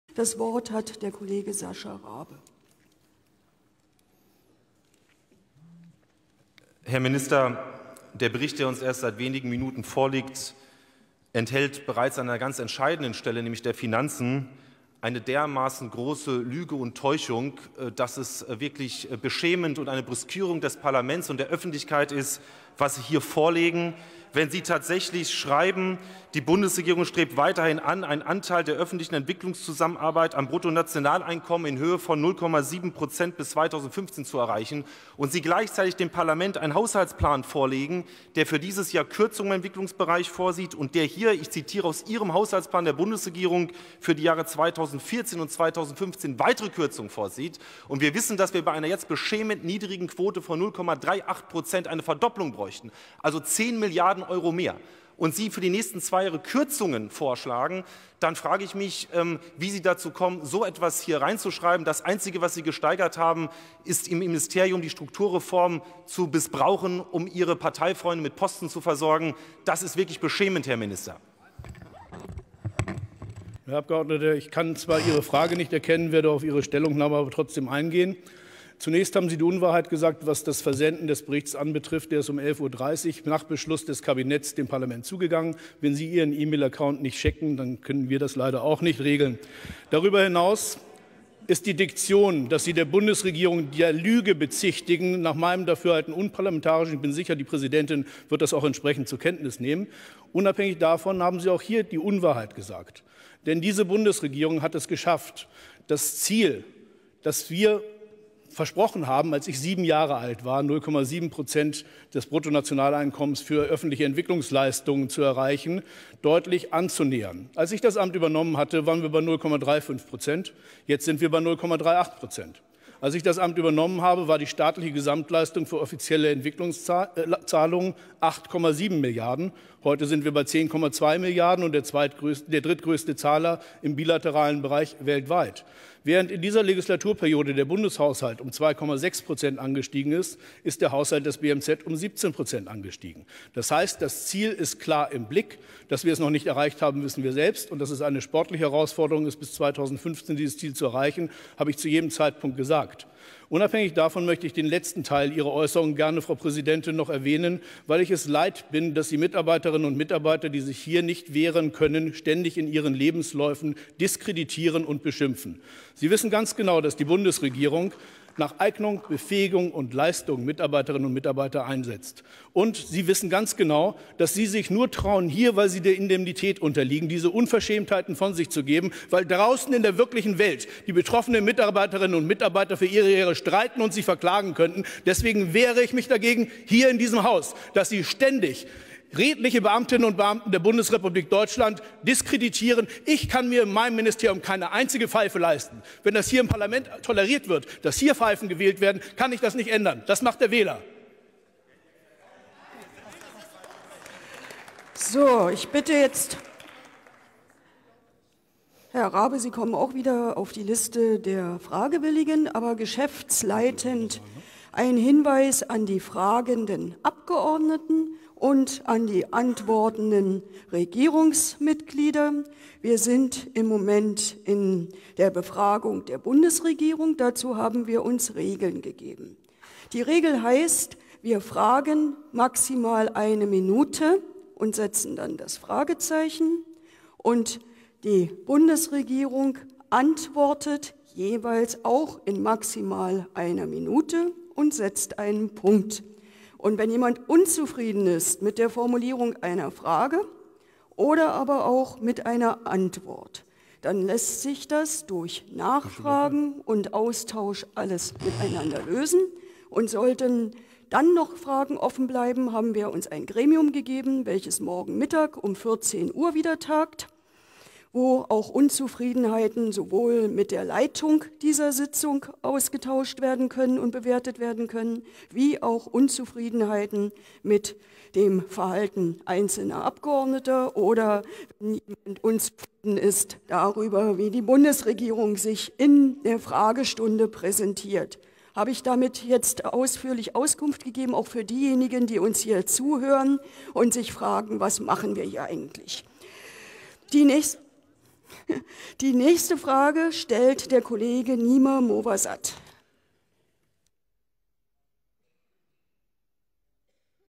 Hitzige Befragung der Bundesregierung (17.04.2013)
Dirk Niebel verteidigt seine Beamtinnen und Beamten im BMZ: 0:02:33

Ein Video der gesamten Befragung der Bundesregierung (Bundesminister Niebel) findet man